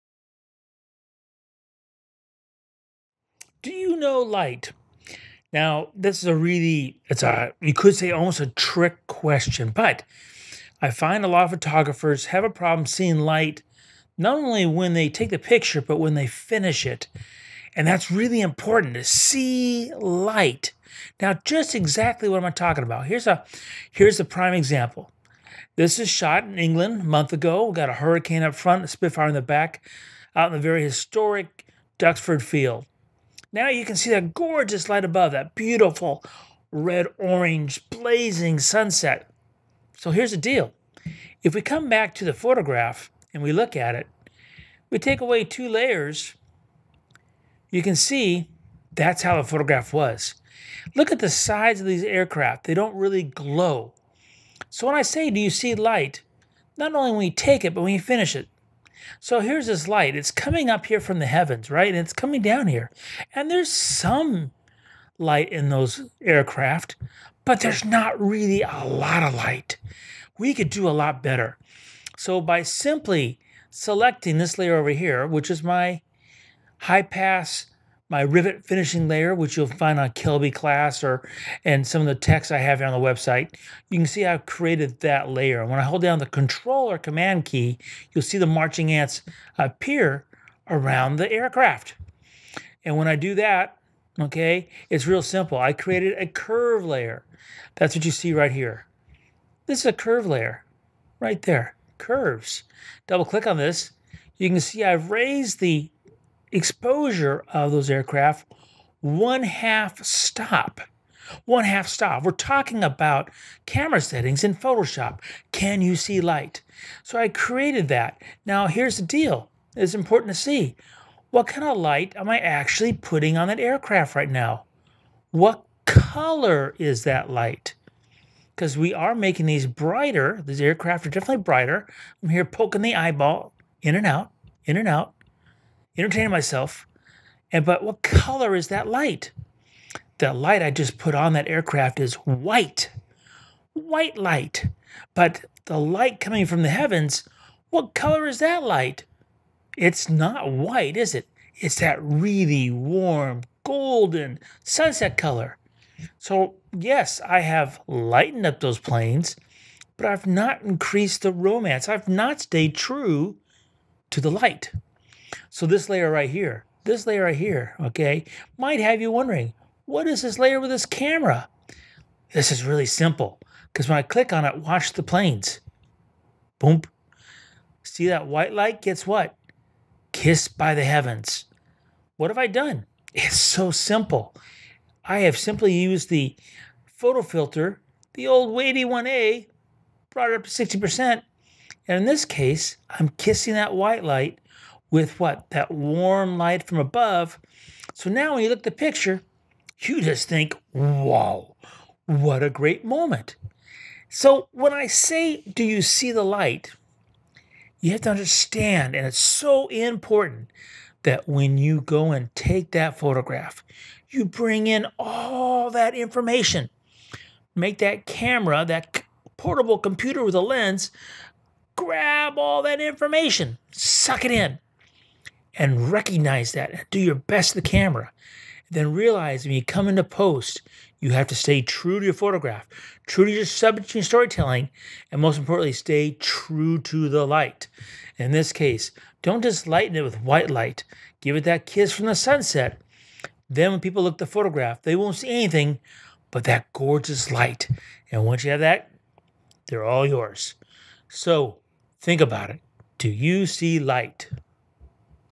You See Light?
Seeing light is such an essential part of photography! But this need goes beyond the viewfinder and the point of capture, it continues on when finishing your images in the digital darkroom. Here's a simple video with some simple techniques and thoughts to help you see light. Remember to head to the website to see the entire blog post.